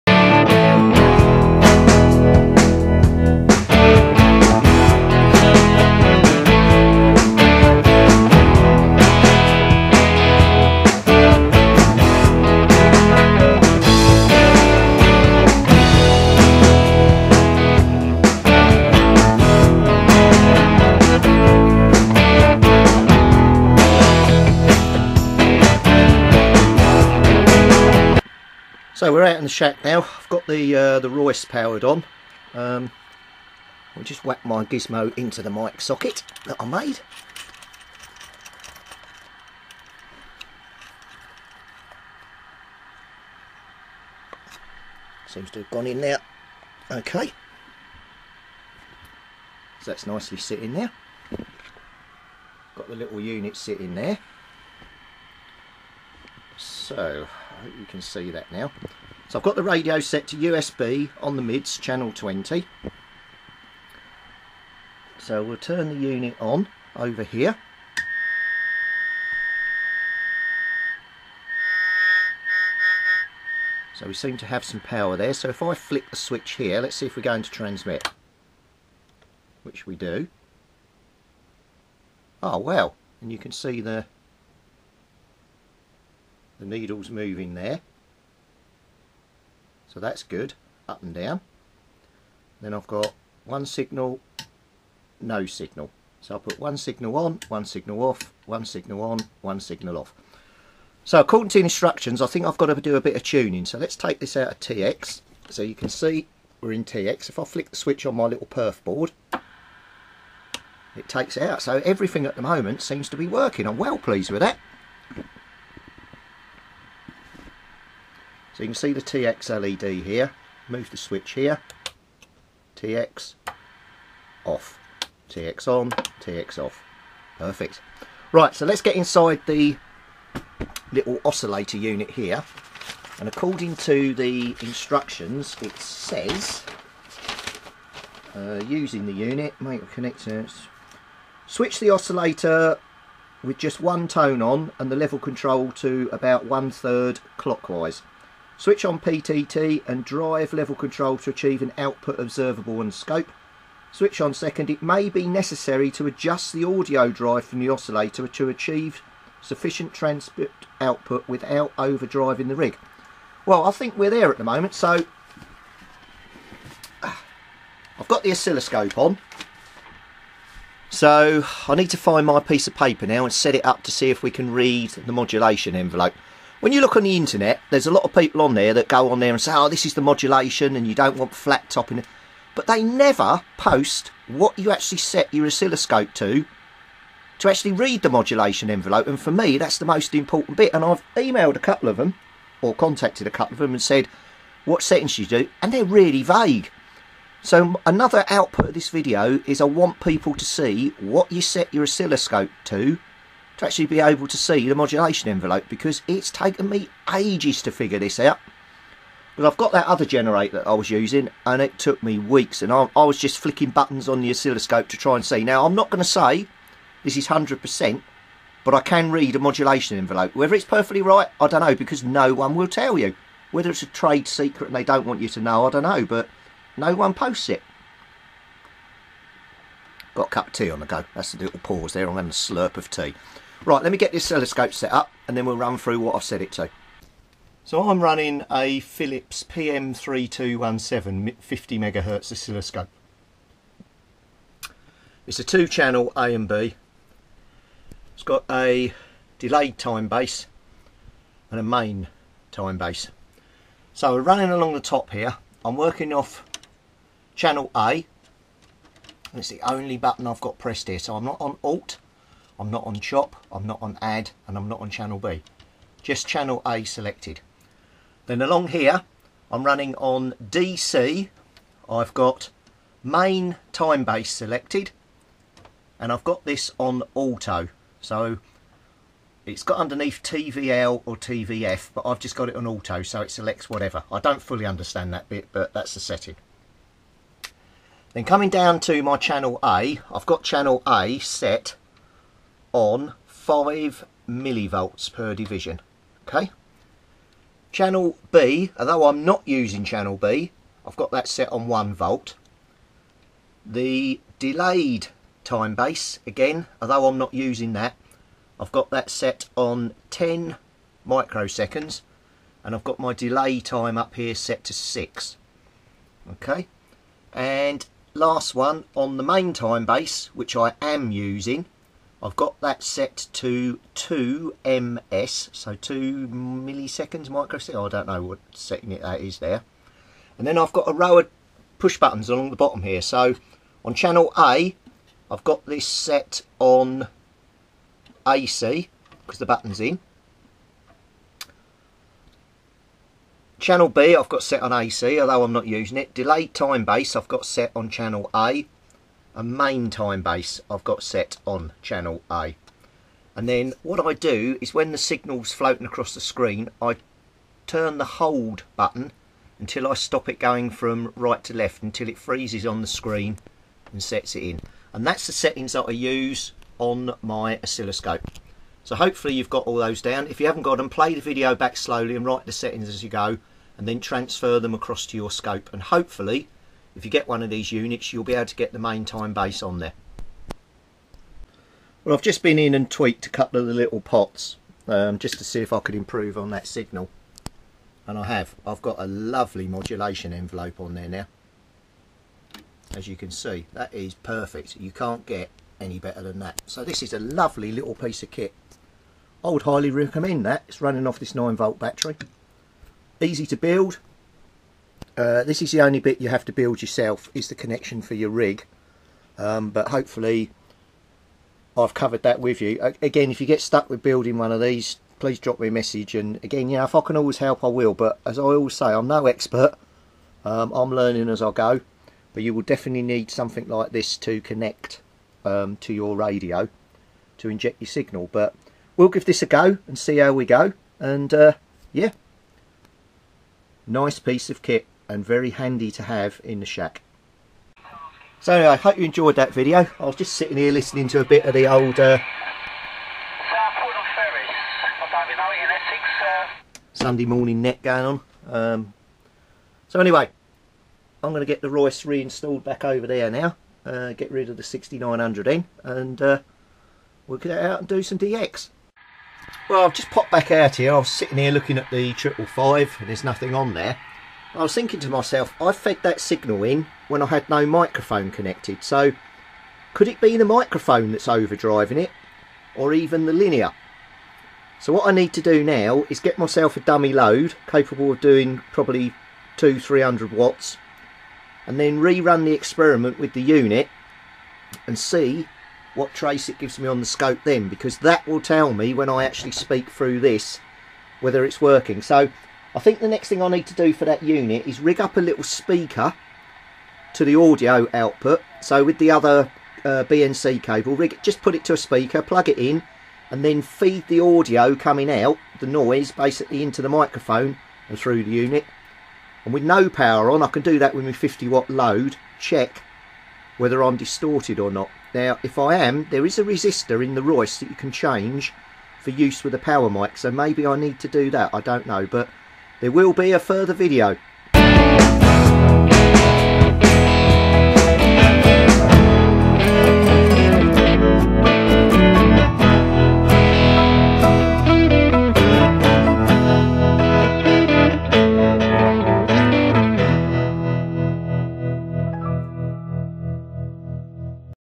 SSB CB Radio Two Tone Oscillator - Part IV
Hi all
The kit ois going welll and today its time to test this thing out, will it work as it should well you will have to watch and see!